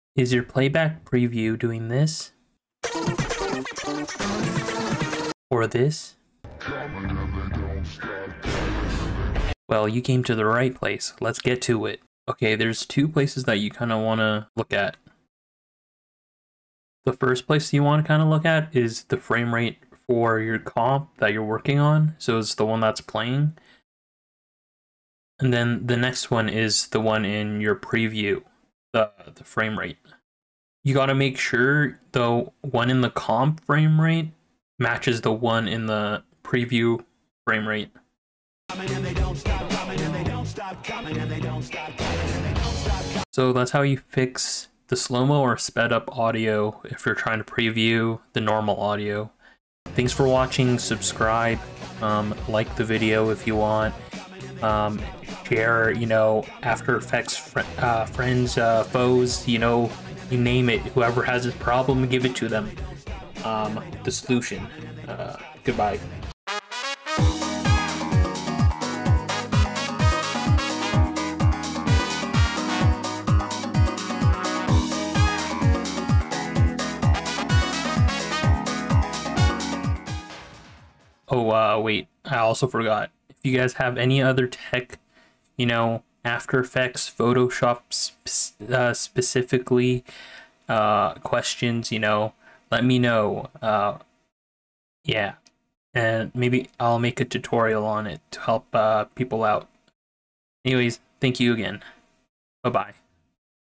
How to fix AUDIO PREVIEW in after effects from sounding SLOW or SPED up.
A tutorial of how to fix the audio preview in after effects from sounding really slow mo or really sped up.